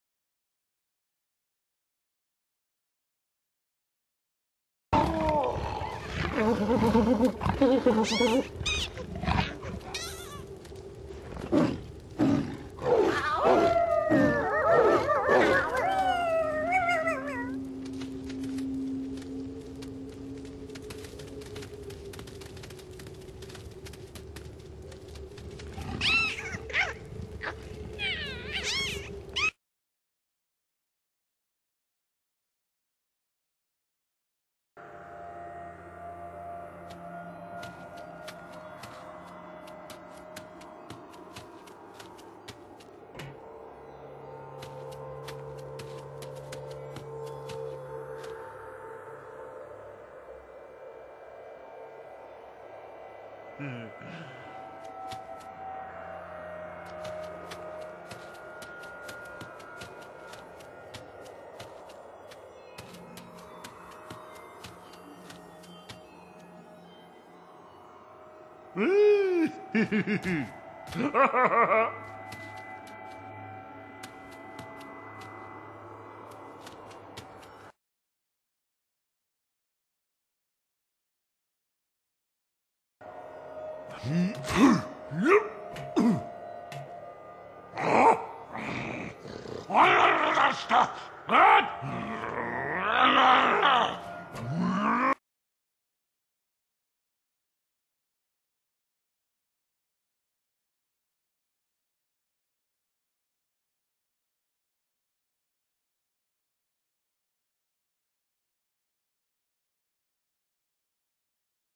The Grox Life Theory
What I think turned the evil demonic but kinda cute Grox into mindless killing machine's...